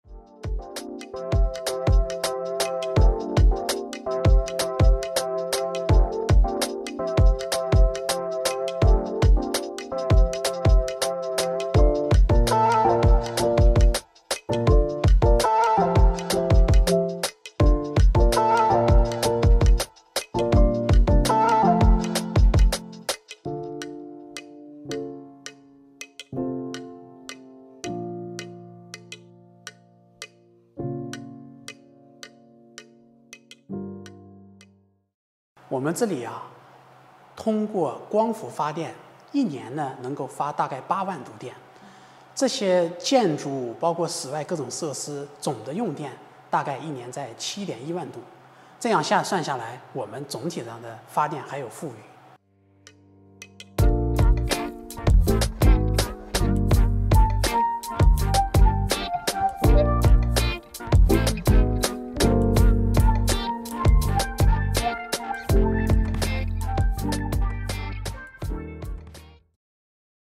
Clean, renewable energy powers low-carbon village in China's Zhejiang
In recent years, many places in China have promoted low-carbon and zero-carbon villages, such as the one in Jiashan County, Jiaxing City, Zhejiang Province. The village was once a ruin, now a zero-carbon village with a typical Chinese water town scenery. The roofs of buildings in the village are covered with photovoltaic tiles to realize solar power generation.